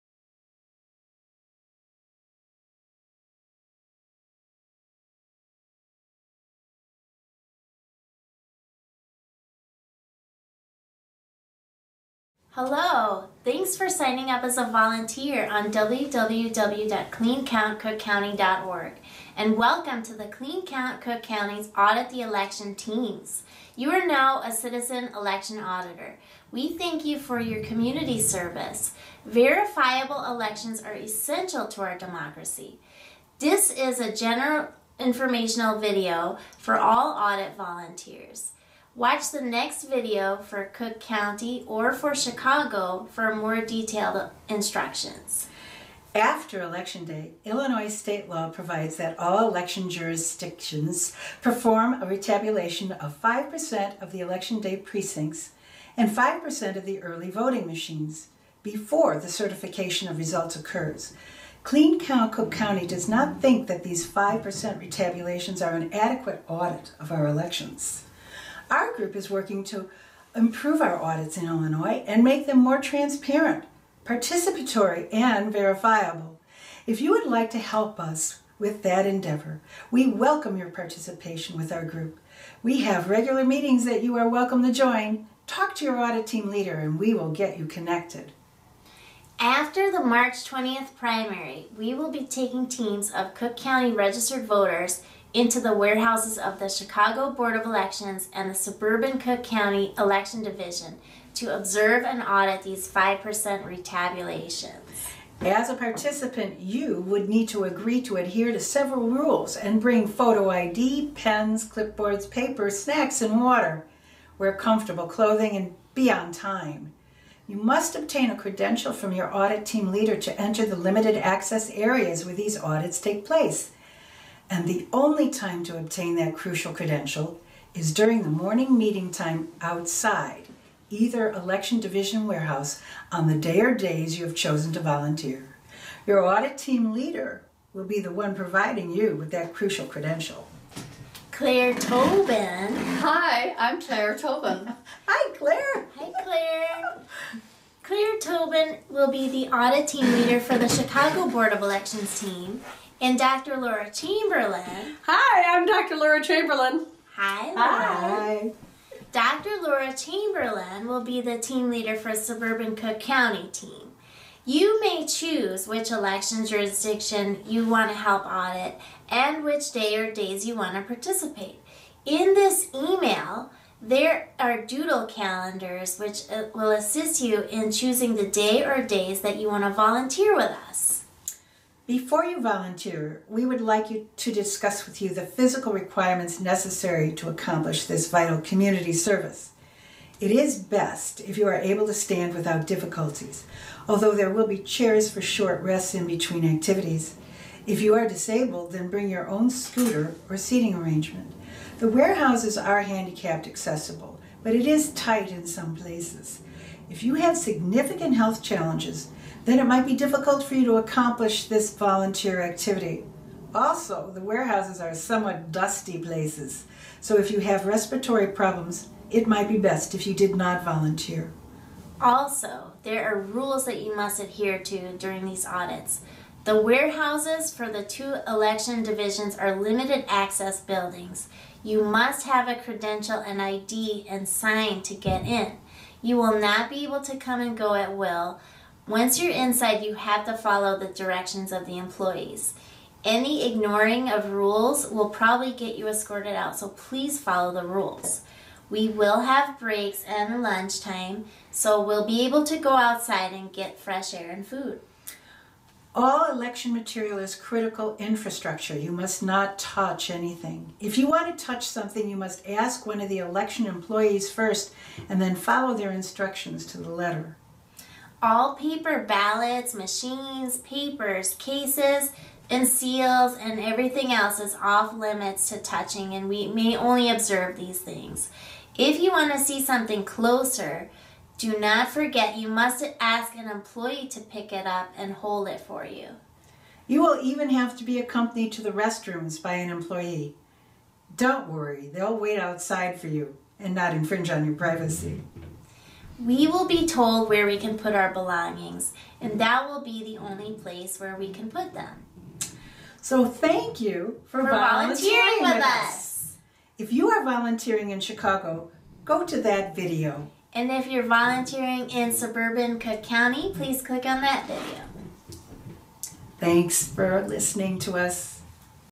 Become a Citizen Audit Monitor! Watch this short Clean Count cook County video!
Make sure your vote is counted in the upcoming elections!  Become an Audit Monitor in Chicago or Cook County to prevent Election Fraud and make sure all ballots are properly counted.  Watch this short video to find out how you as citizen activist can help protect the vote count and our democracy. Clean Count Cook County.org is  citizen action group advocating Transparent, Fair and Accurate Elections in Chicago and Cook County.  We invite citizens to OCCUPY ELECTIONS to take charge of our voting system!